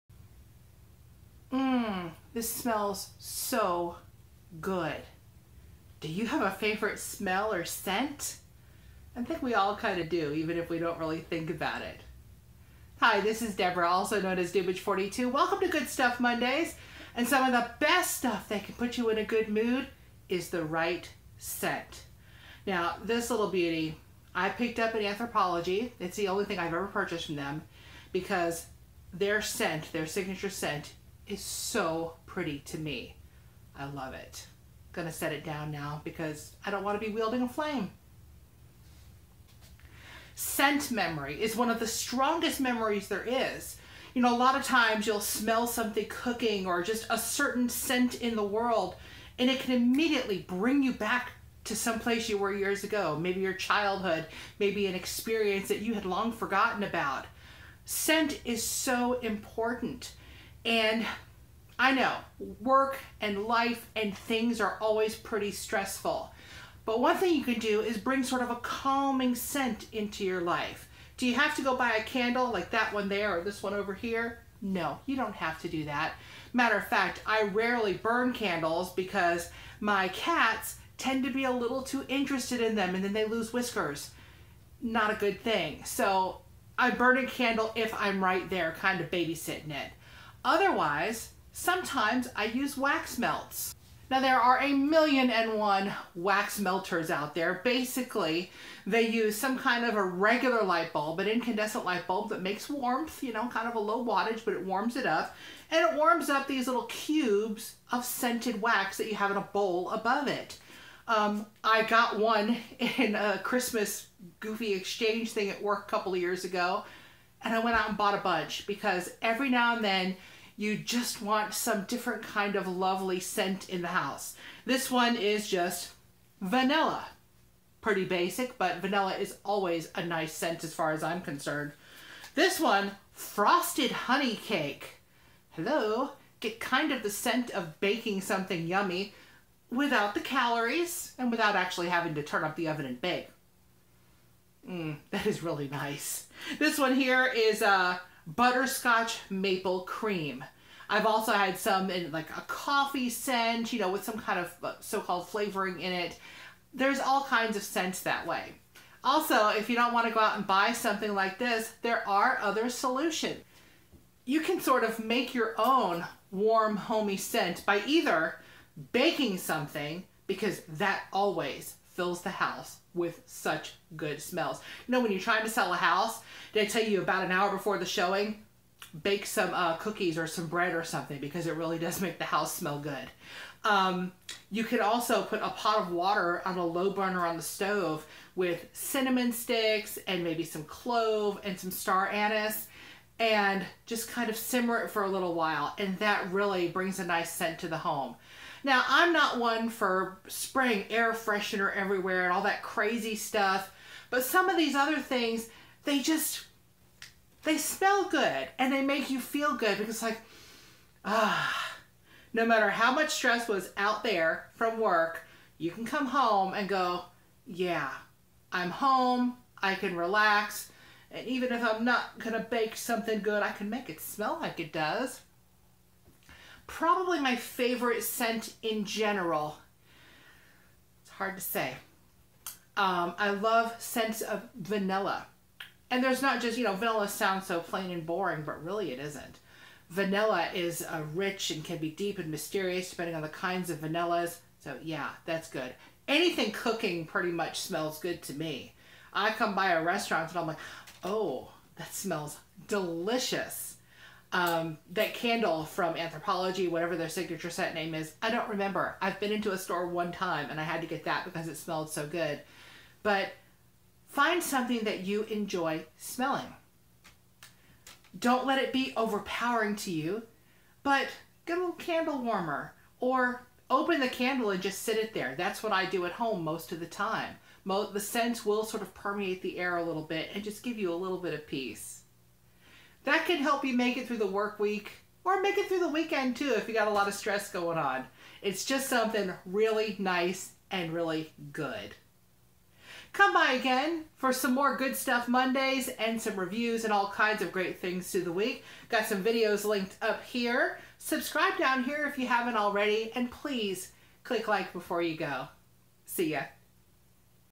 Mmmm... That smells so good! - Good Stuff Mondays
The right scent can change your whole mood - and help you release some stress.
Here are a few things I like to do to bring some relaxing nice scents into my day.

I upload Monday, Tuesday, and Thursday each week.

Also, come visit me on my other social media.

See ya! :)